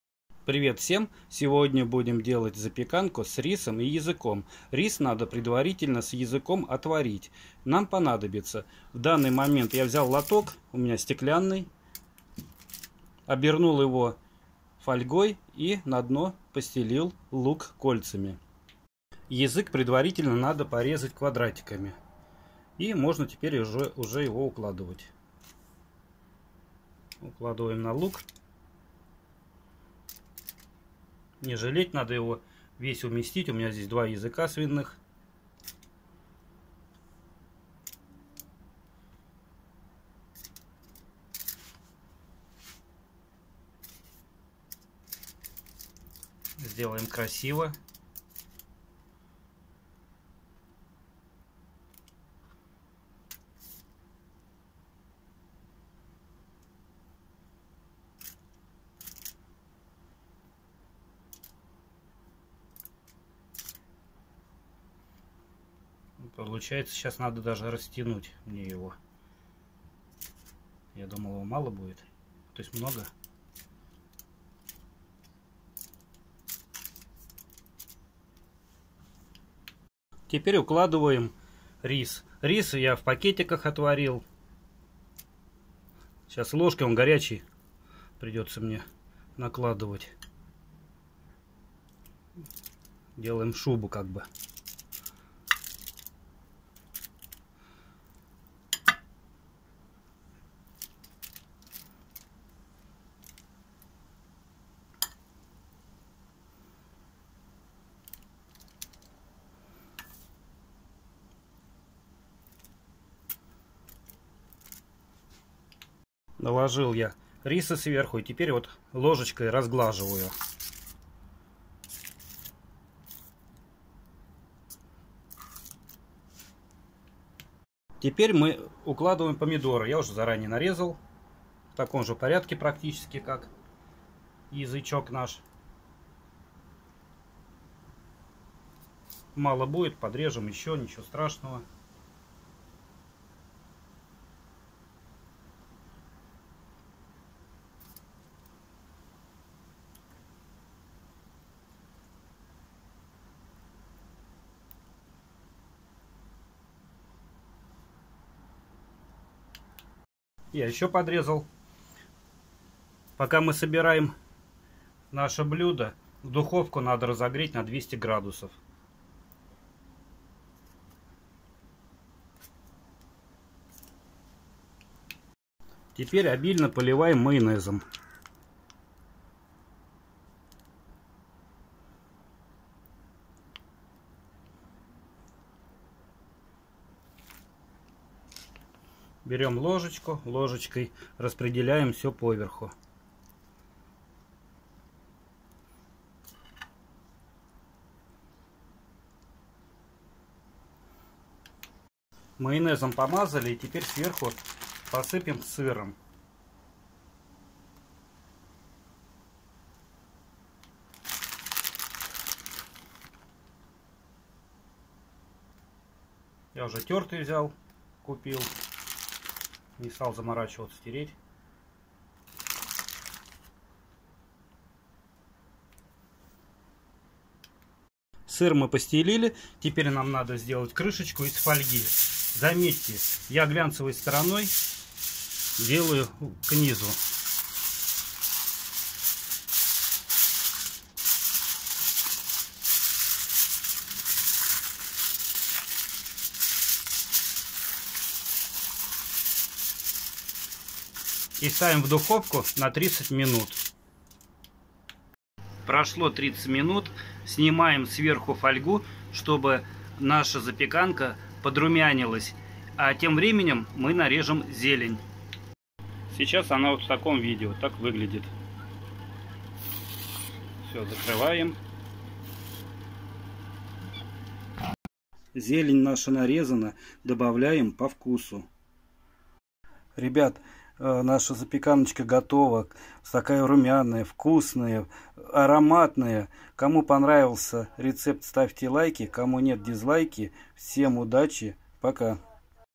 Рецепт запеканки с рисом и языком. Вкусно и просто.
Запеканка с рисом и языком. Вкусно и просто.
Привет всем, сегодня будем делать запеканку с рисом и языком.
Рецепт:
Рис отваренный в пакетиках.
Свинной язык отваренный.
Лук.
Помидоры.
Майонез.
Сыр тёртый.
Зелень.

Духовку разогреть на 200°
Ставим в духовку на 30 минут.
Приятного аппетита!